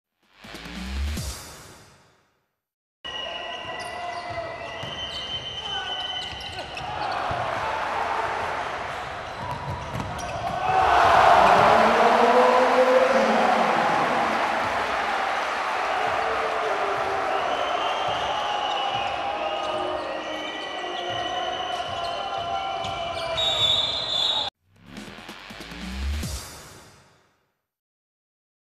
Dario Drežnjak blocks, Mateo Drežnjak dunks (Široki - Borac, 28.11.2019)
Drežnjak brothers on a rampage! Dario blocks, Mateo dunks on fast break!